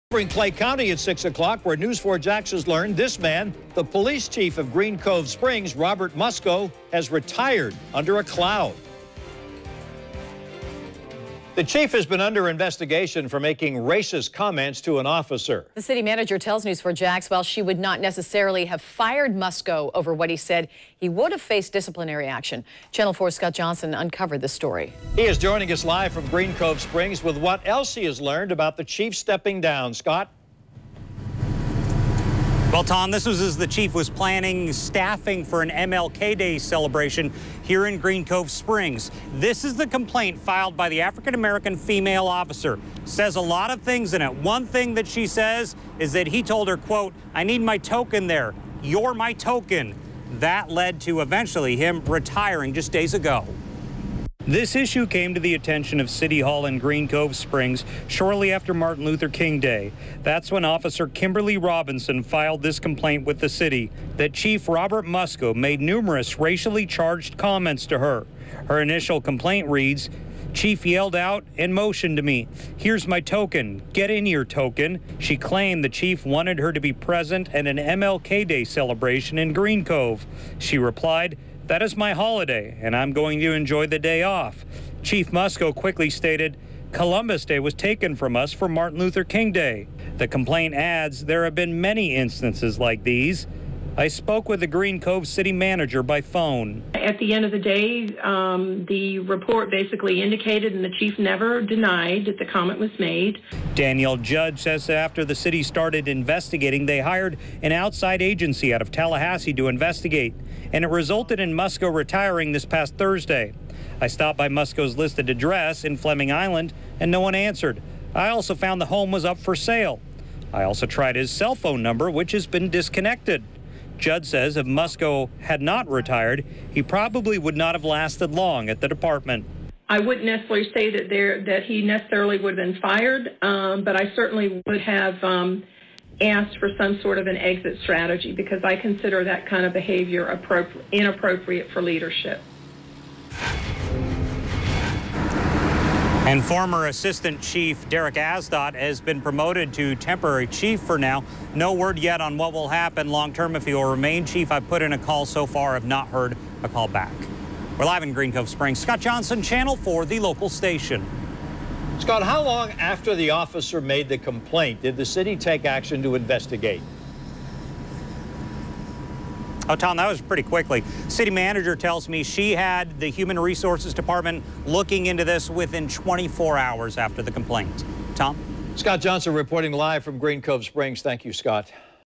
Police Chief retires during investigation
VIDEO: The Green Cove Springs Police Chief, who was under investigation for making racist comments to an officer, has retired.